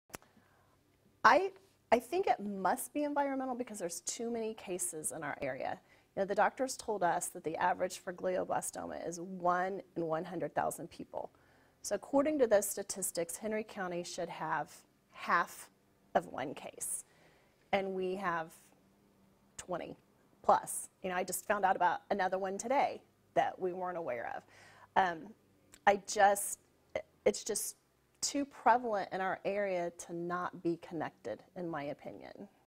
Woman describes the number of cases of rare brain cancer in Henry County
State health officials meeting with families today